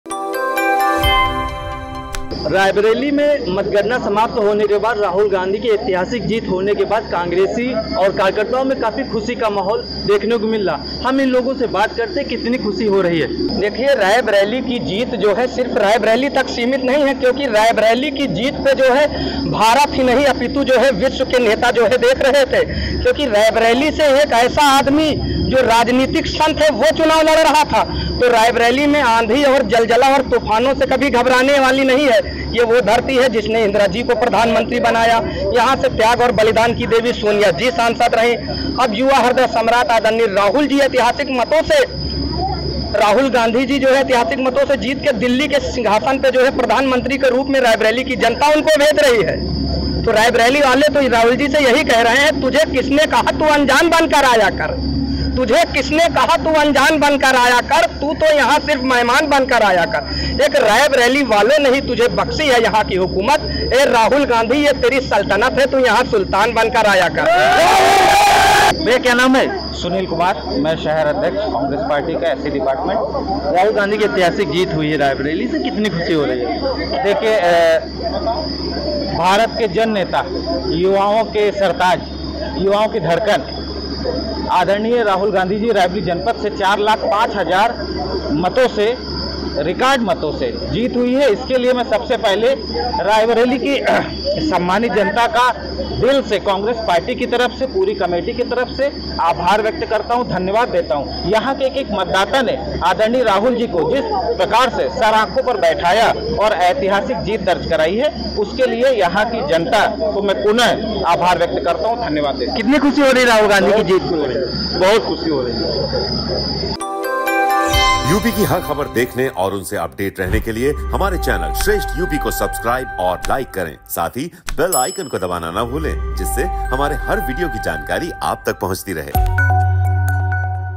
Loksabha Election Results 2024: Raebareli में Rahul Gandhi की जीत पर कार्यकर्ताओं में जश्न
Loksabha Election Results 2024: रायबरेली में राहुल गांधी (Rahul Gandhi) की जीत के बाद कांग्रेस में जश्न का माहौल, कार्यकर्ताओं ने कहा, ये केवल रायबरेली की जीत नहीं पूरे भारत की जीत है, अब राहुल गांधी पीएम बनने जा रहे हैं.